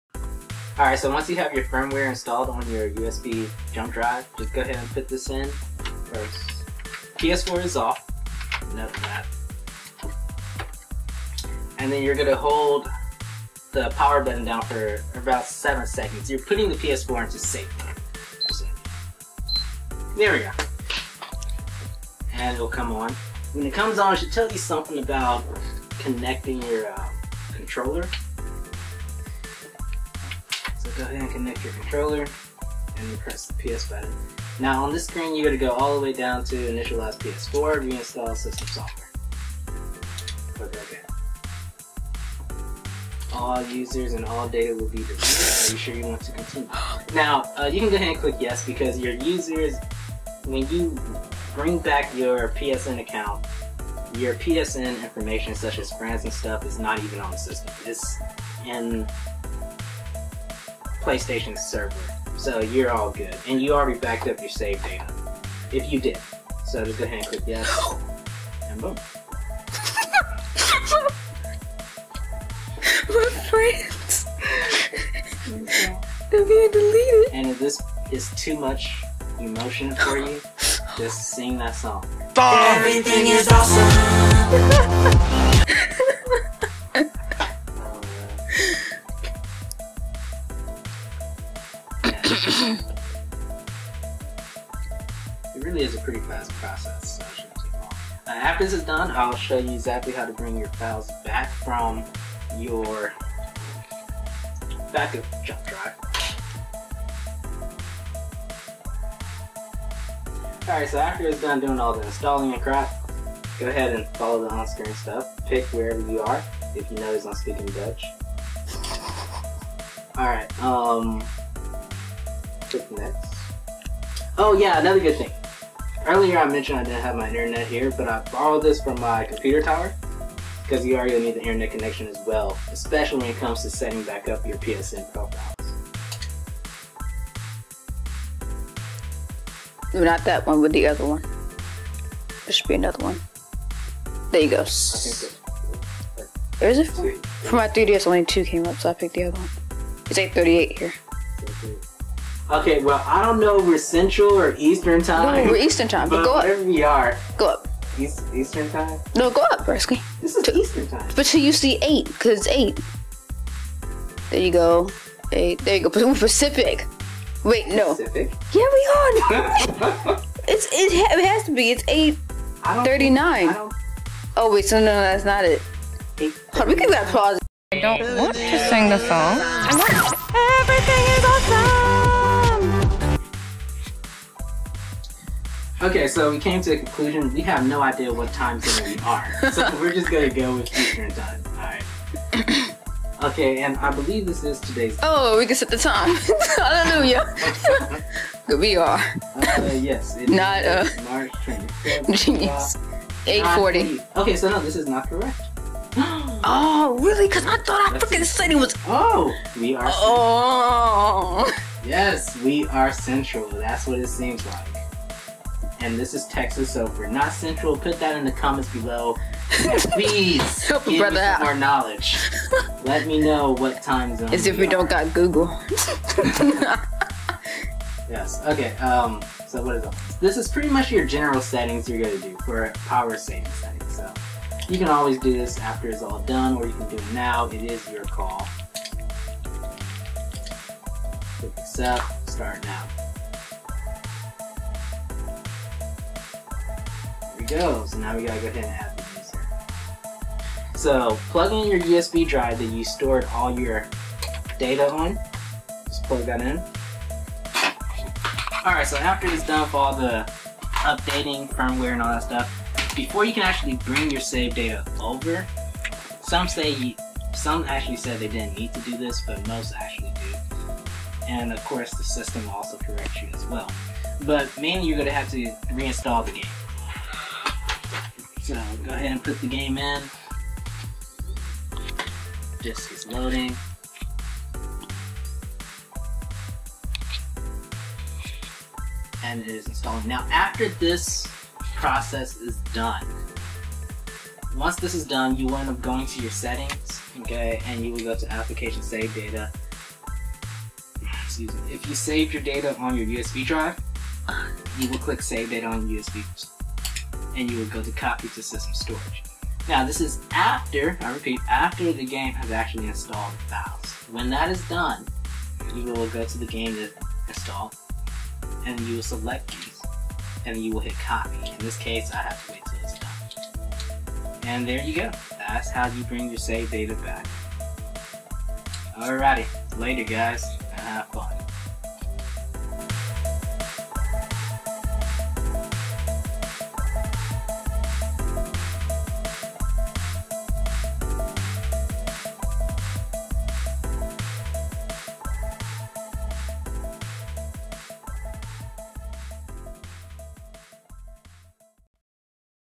PS4 Hard Drive Upgrade Part4 - Installing PS4 System Firmware: Xskode
In a world where games become better with graphics and add ton of content, there is only one thing that matters... Making Sure You Have A Lot Of Space To Save Your Games!

Join us in this exciting moment of increasing our PS4 Storage capacity!